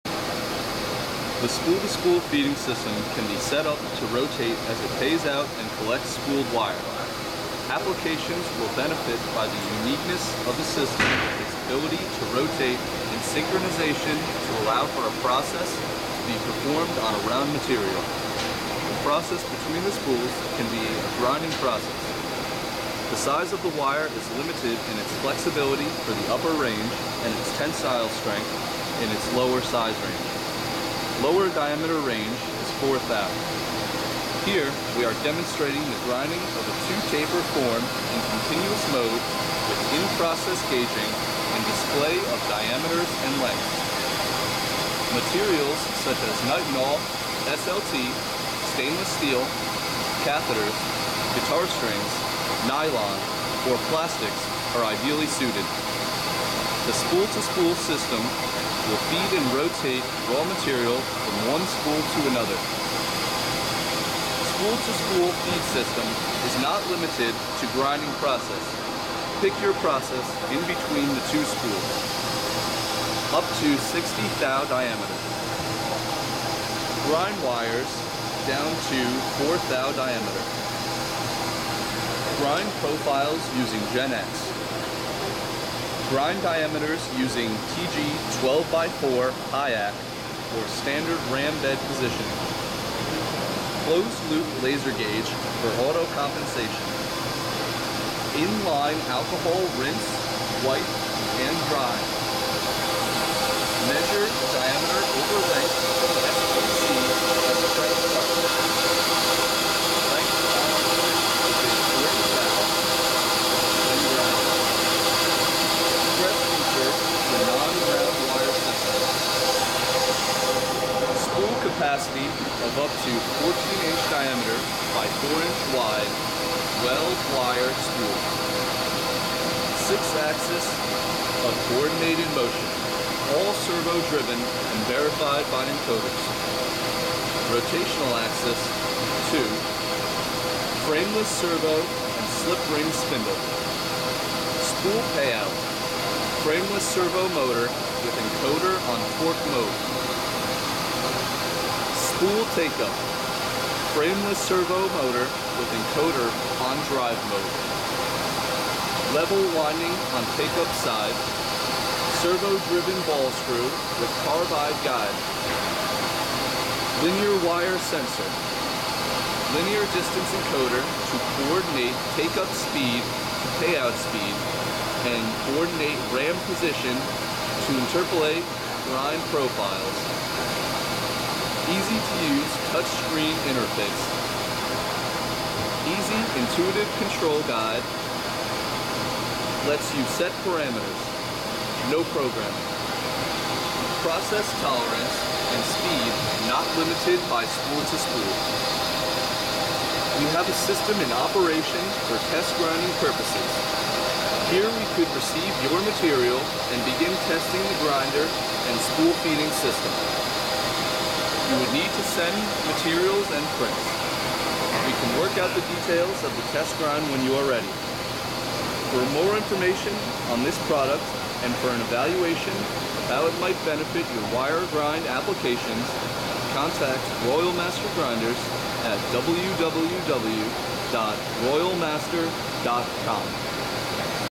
Royal Master Spool to Spool Grinding system
The Spool to Spool centerless feeding system is a revolutionary patented system designed to feed material from a spool through a machine and wrap back up on another spool. the spools rotate upwards of 3500 rpm and have coordinated servo motion. many different processes can be in the middle of the two systems including guidewire grinding or simple thrufeed grinding to remove oxide off the outside layer of NiTi